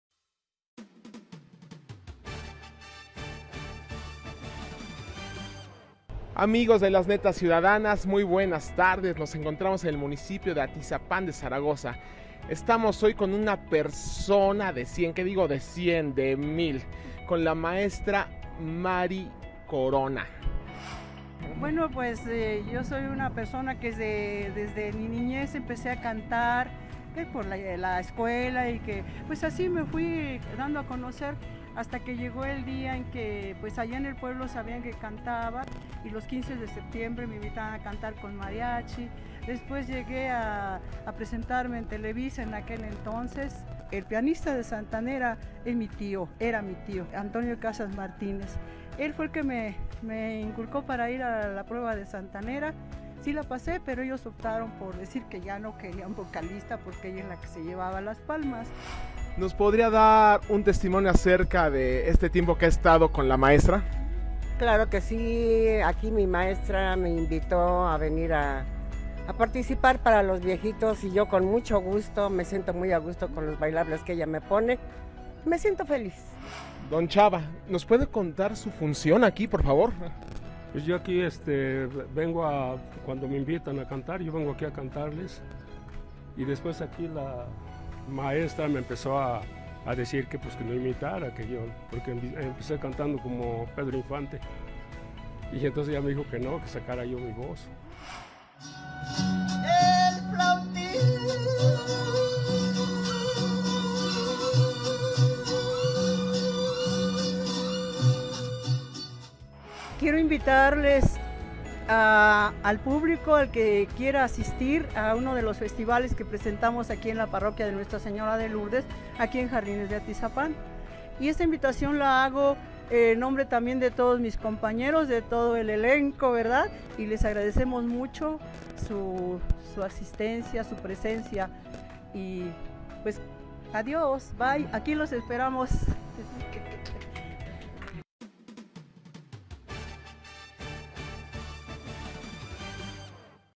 1 DE 100 "CANTANTE" | NETAS CIUDADANAS
Doña Mari, nos contó sus netas y amor por el canto. Ella es una mujer de cien, ¡conoce su historia!